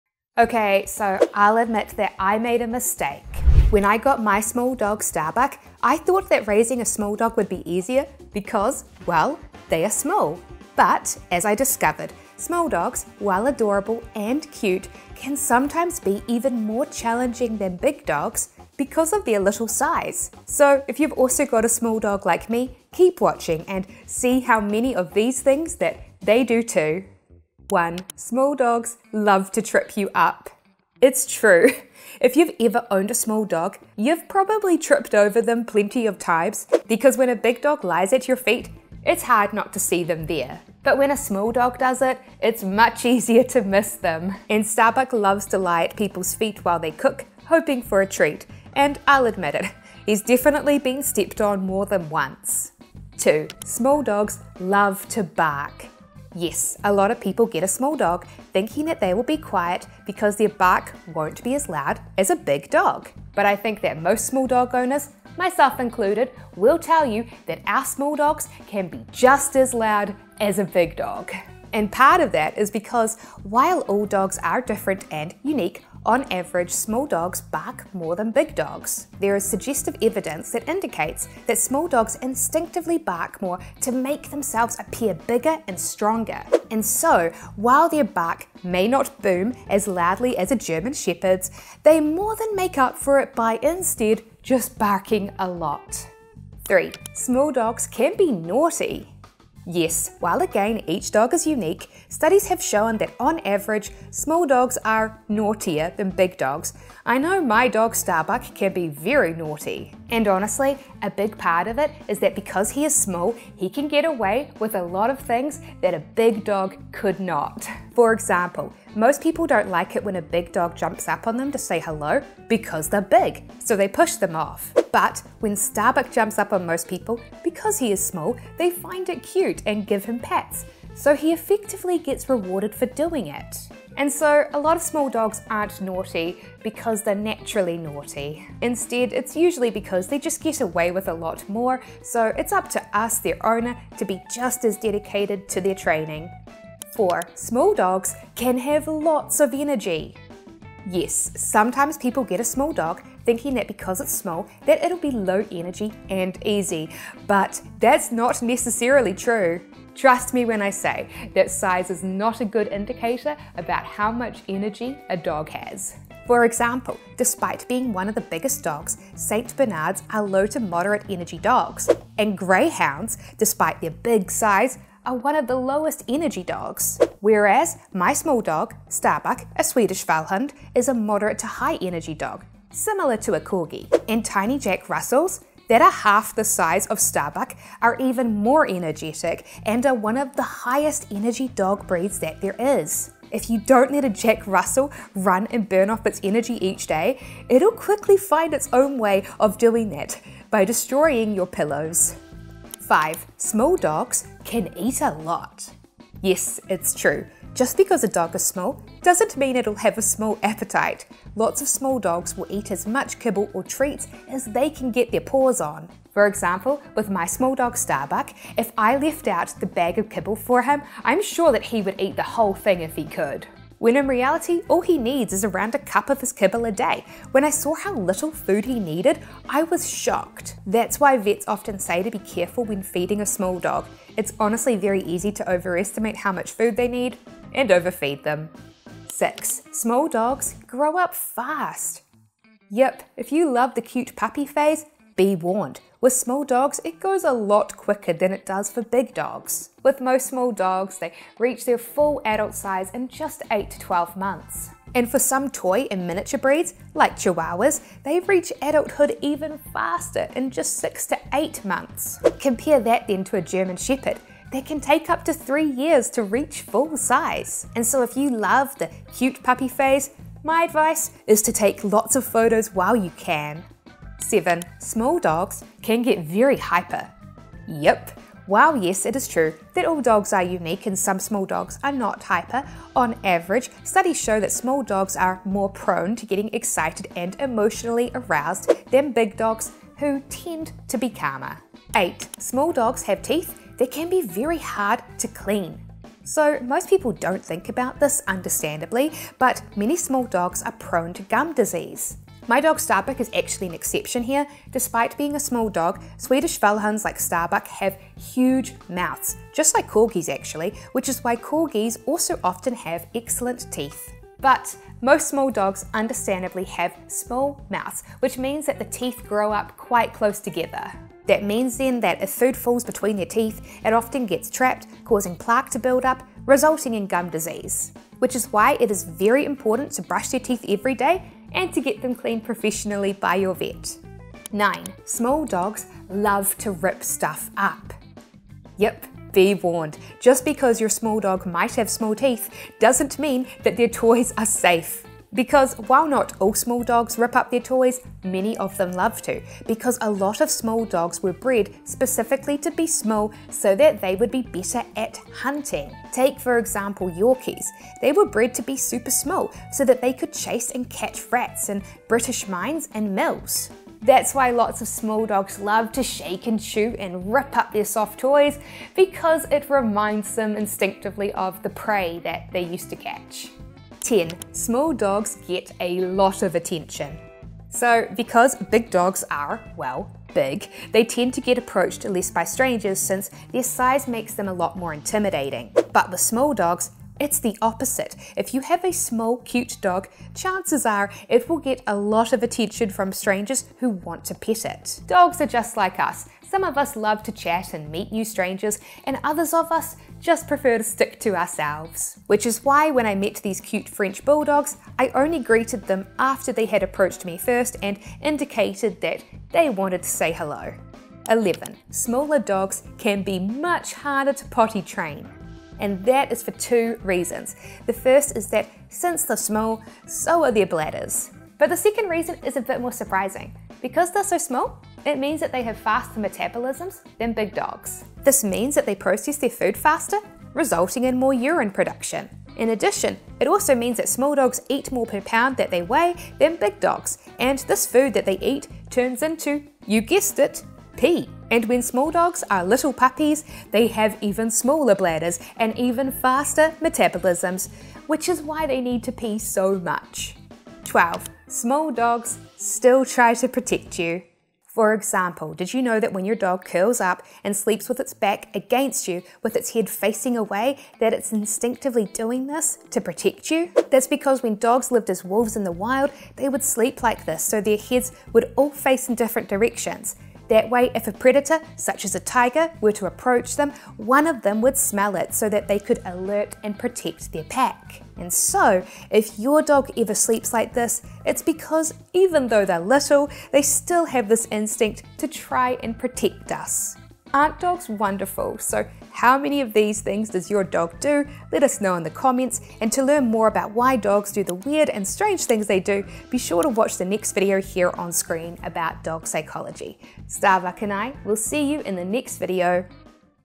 12 Things NOBODY TELLS YOU About Small Dogs
Yes, it's true: small dogs can be cute and funny, but also challenging! How many of these does your dog do?

Timestamps:
0:00 Why I Made A Mistake
0:27 Small Dogs Love To Trip You Up!
0:53 Small Dogs Love To Bark!
1:36 Small Dogs Can Be Naughty!
2:27 Small Dogs Can Have Lots Of Energy!
3:22 Small Dogs Can Eat A Lot!
3:59 Small Dogs Grow Up Fast!
4:34 Small Dogs Can Be Very Hyper!
4:52 Small Dogs Have Teeth That Can Be Very Hard To Clean!
5:43 Small Dogs Love To Rip Up Toys!
6:14 Small Dogs Get A Lot Of Attention!
6:51 Small Dogs Can Be Hard To Potty Train!
7:31 Small Dogs Still Try To Protect You!